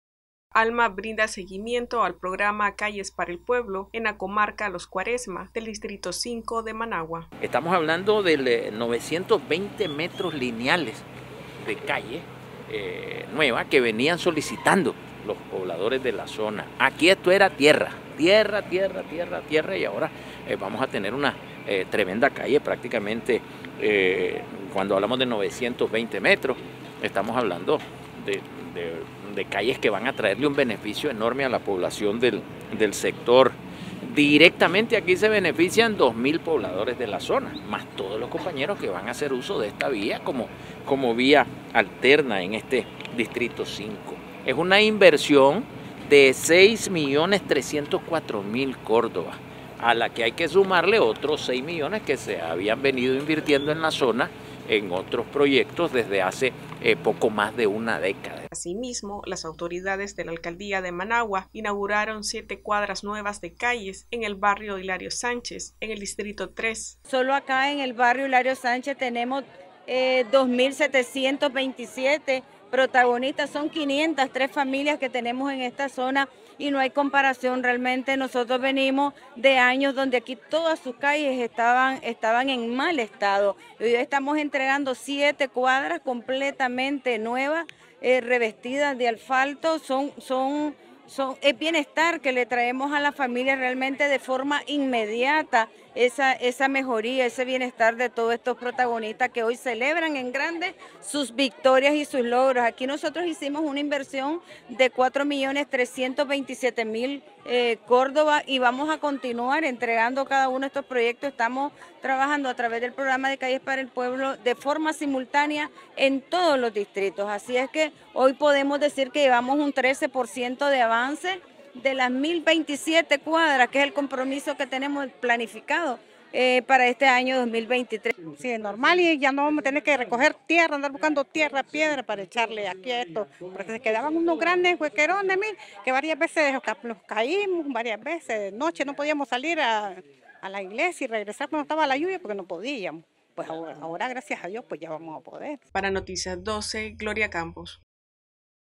Noticias12Nicaragua Alma inaugura 7 cuadras nuevas en el barrio Hilario Sánchez
Alma inaugura 7 cuadras nuevas en el barrio Hilario Sánchez